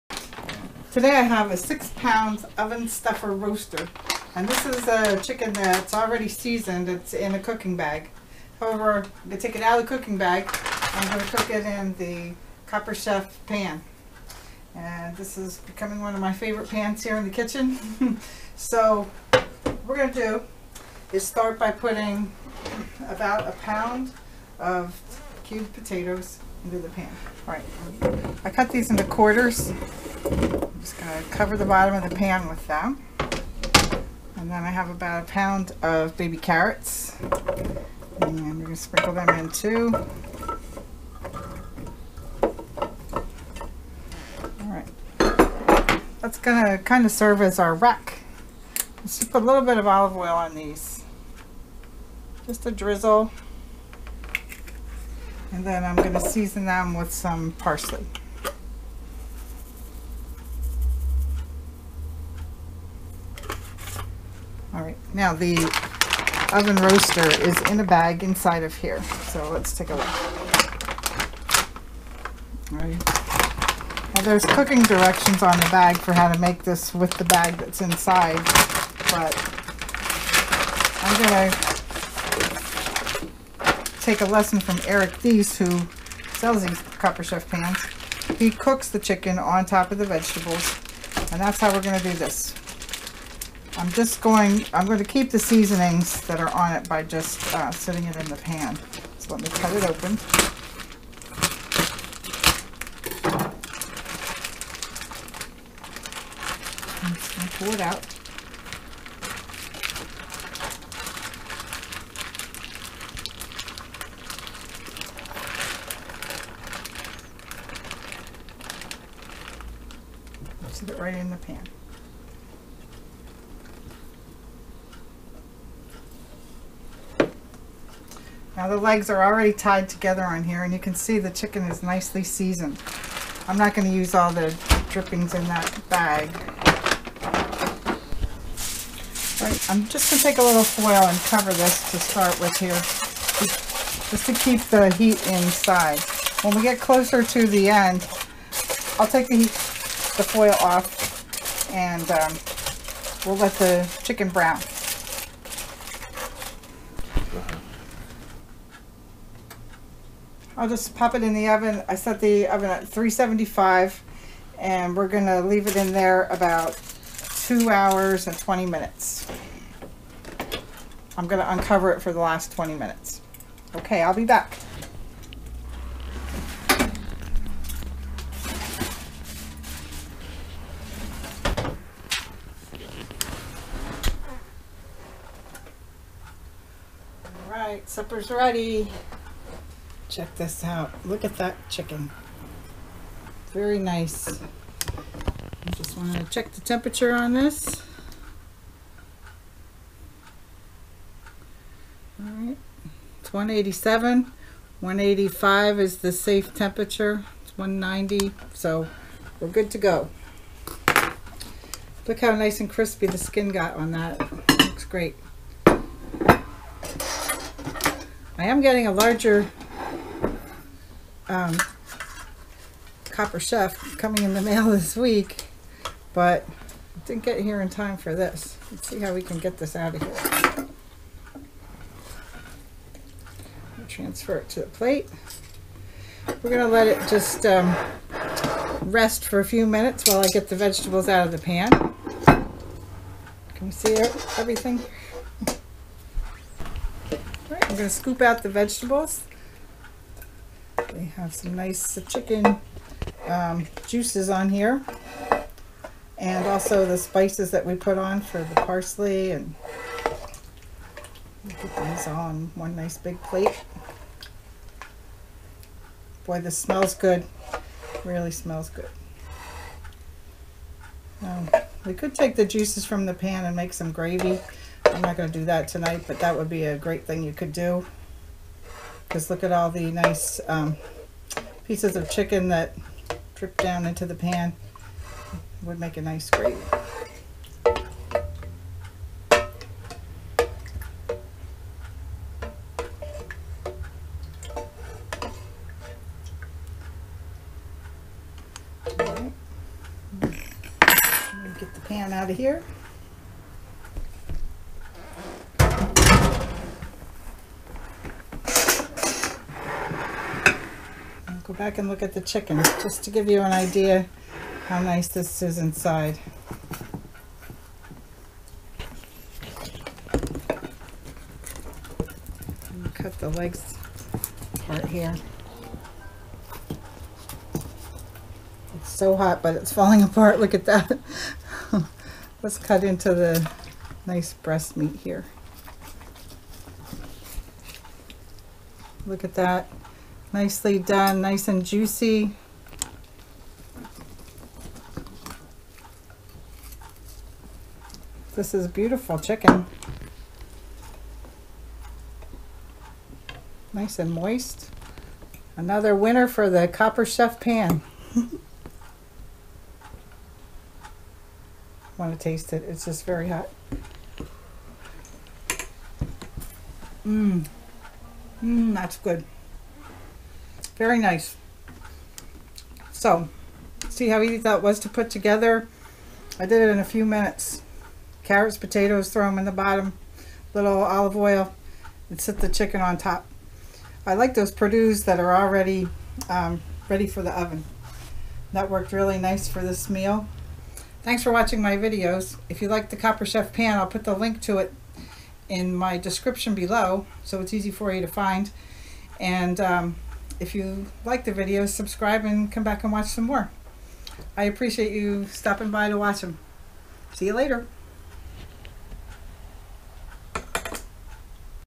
Roasted Whole Chicken in Copper Chef Pan
You can get this Copper Chef Pan
I roasted a whole chicken in the oven, on top of potatoes and carrots.  This was a Perdue oven roaster that was already cleaned and seasoned.  So simple.  So delicious.   I followed Eric Thiess's method, using the Copper Chef pan.  Winner Winner, Chicken Dinner!